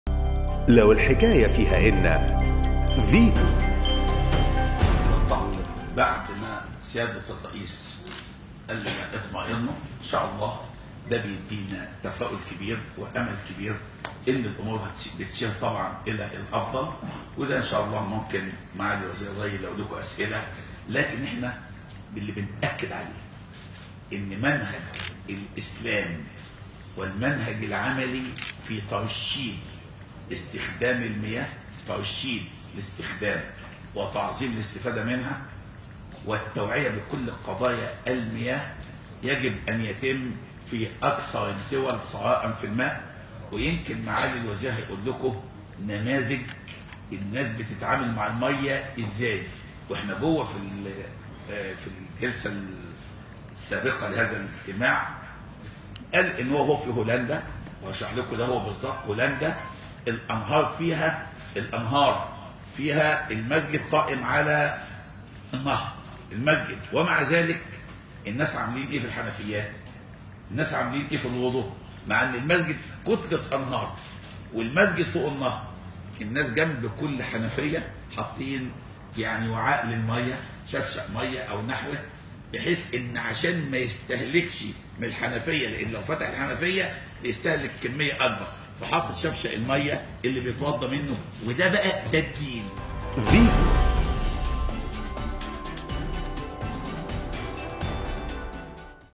وزير الأوقاف: في هولندا يستخدمون «الشفشق» لترشيد استهلاك المياه
فيتو -  وزير الأوقاف: في هولندا يستخدمون «الشفشق» لترشيد استهلاك المياه
ـــــــــــــــــــــــــــــ